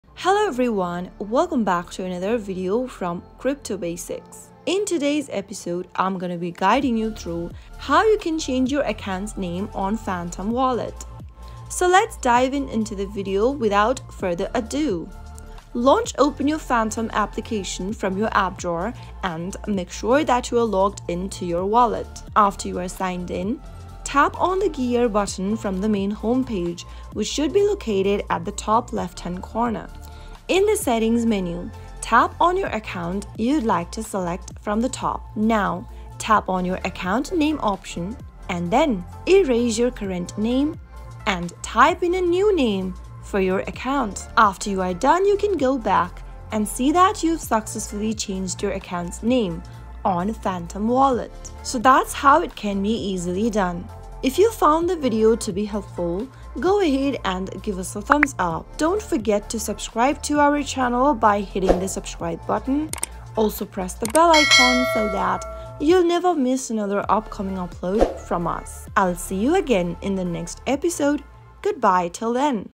How to Change Your Account Name on Phantom Wallet 2023?
Make your Phantom wallet truly your own by customizing your account name. This video will show you how to easily change your account name in Phantom wallet, giving your crypto identity a unique touch.